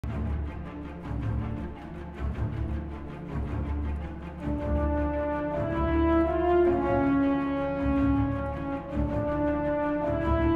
lego Star Wars: IG-11 ( black and white )
here is another video of ig-11 but in black and white, so i hope you all really enjoy this video